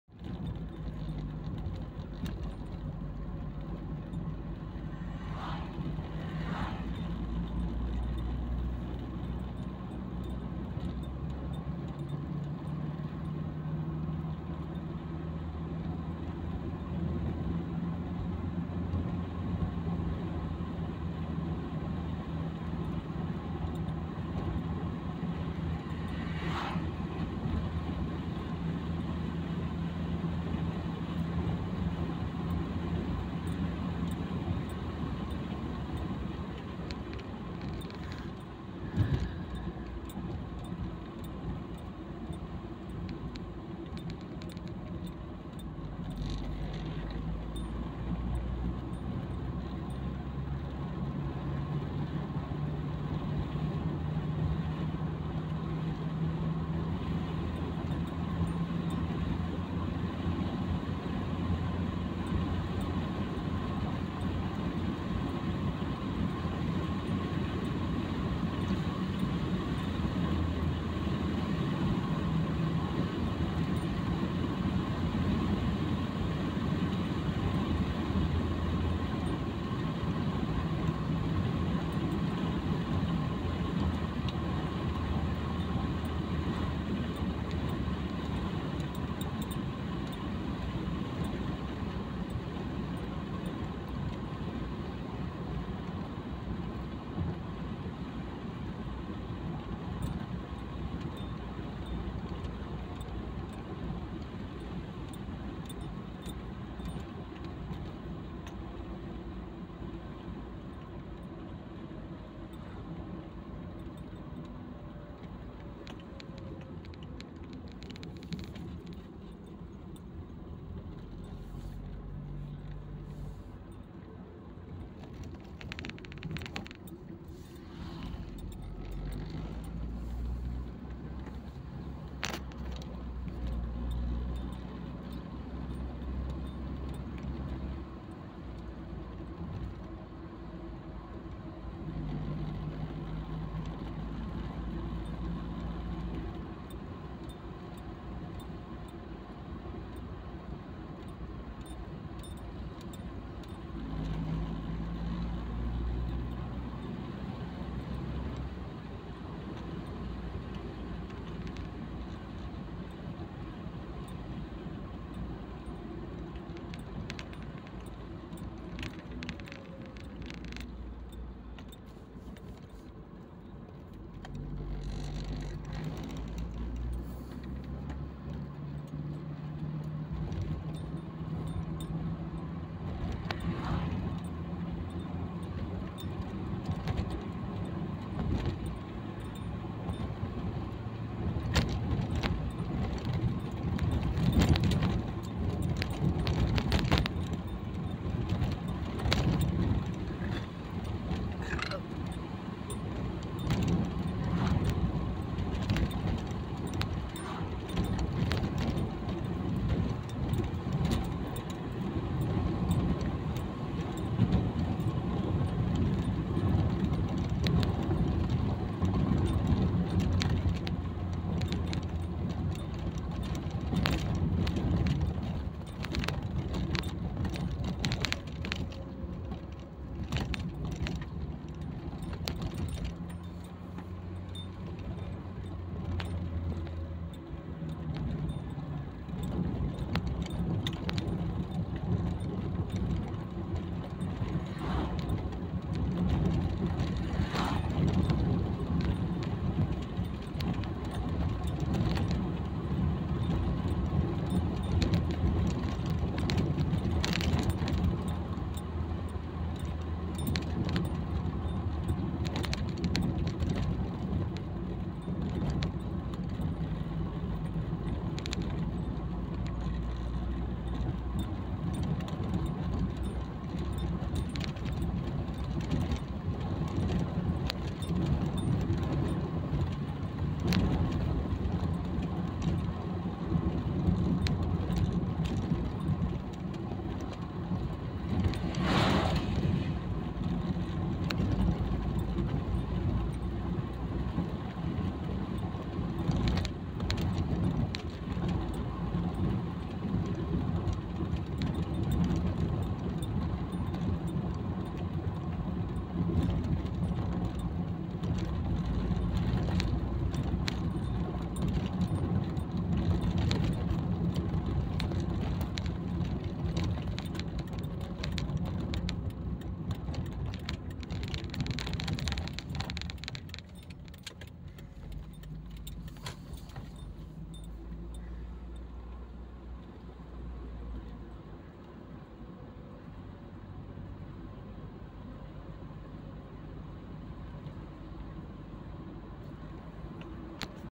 Всем доброго дня и хорошего настроения, пусть Господь хранит вас на всех ваших путях
В этом видео показана дорога от Старых Радулян во Флорешты в Молдове в летнее время, а вот теперь выпал снег и дорога заснеженная, так что цените летнее время